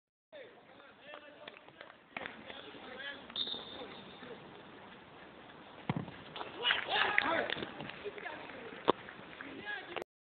Penalty Jimmy contre Goal Foot
But sur Penalty de Jimmy pour Saint Denis contre Goal Foot